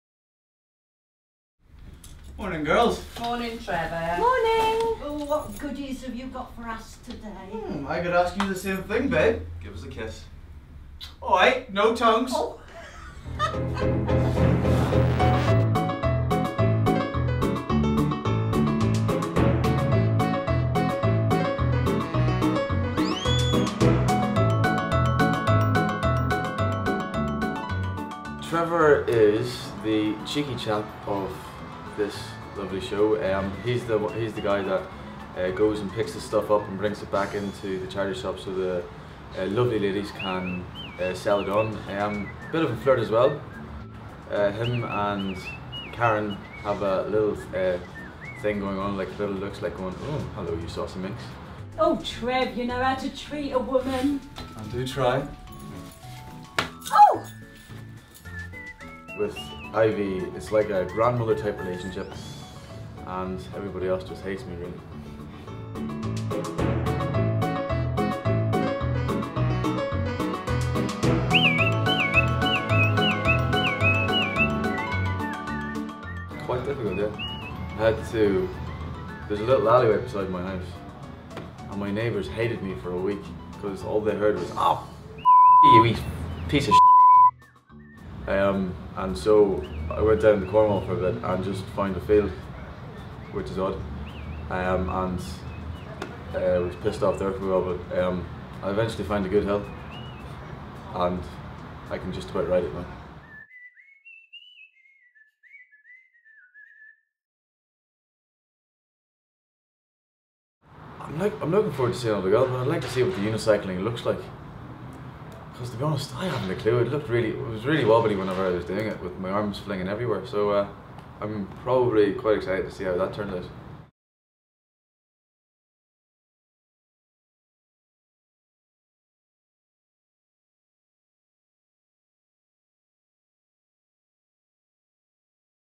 Help Yourselves Behind the Scenes: Trevor
An Interview with Michael Redmond who plays 'Trevor'